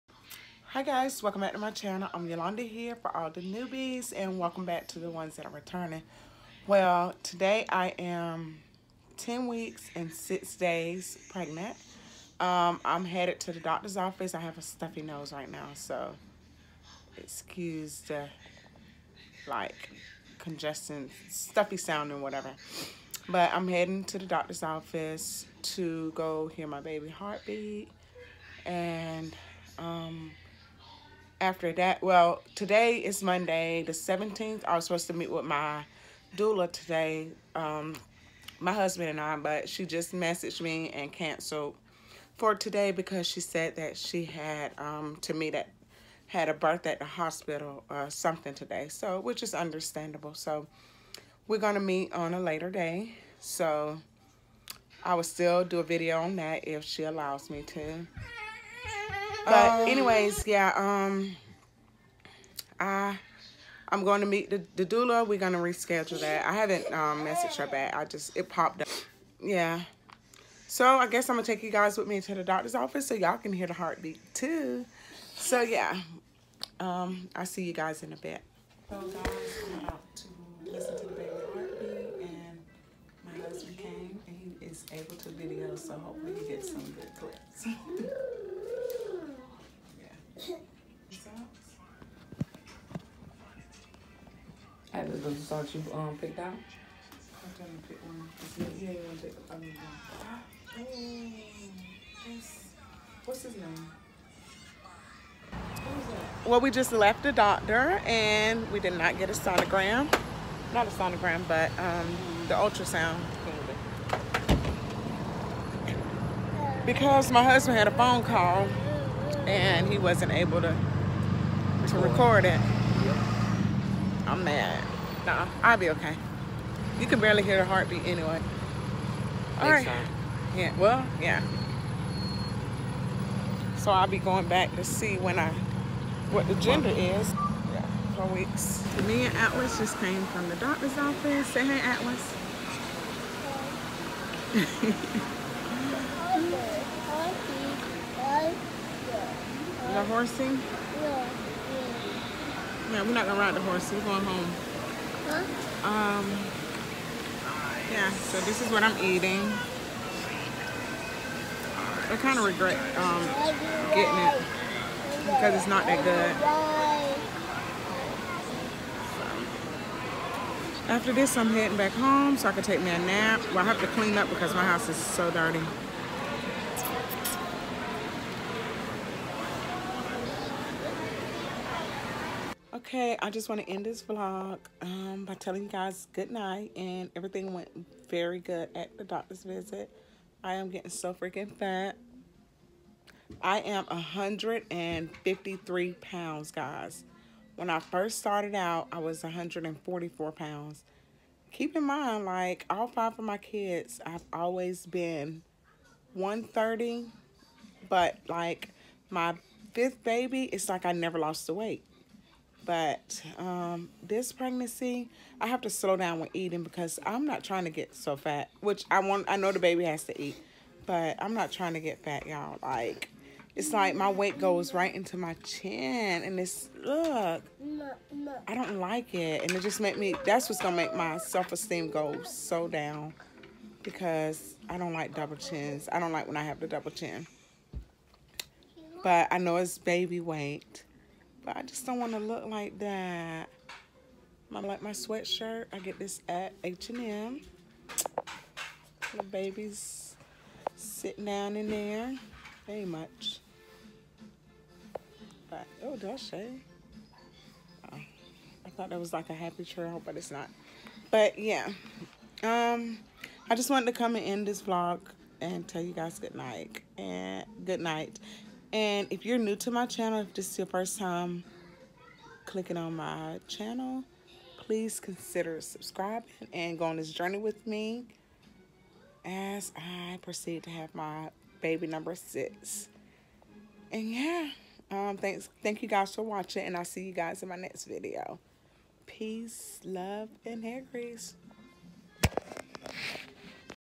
Baby’s heartbeat 💗 10 weeks 6 days
We didn’t get to actually video the sound being that my husband had to step out of the room for a phone call🙄 hopefully my next visit I’ll get better clips.